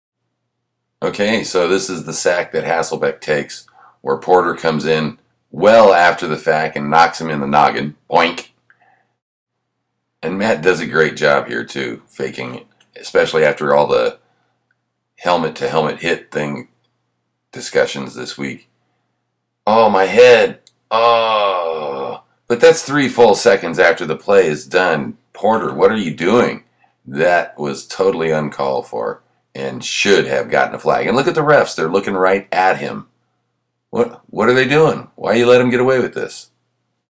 Hasselbeck Sacked
Matt Hasselbeck is sacked by Daryl Washington of the Arizona Cardinals.  Perennial Seahawk thorn Joey Porter then plows into Hasselbeck 3 full seconds after Matt is down.  No flag.  Refs looking right at the situation.

WTF is Joey Porter thinking here, anyway?  Oh, that's right...Joey Porter DOESN'T think.  And why were there no flags?  Especially considering all the talk in the NFL about concussions the week leading up to this game.

It's all good.  Hasselbeck was overacting a bit, probably spurned on by his recent appearance on Comedy Central's South Park.  But also a wise veteran move to try to get a flag by grabbing his forehead hoping to get the flag.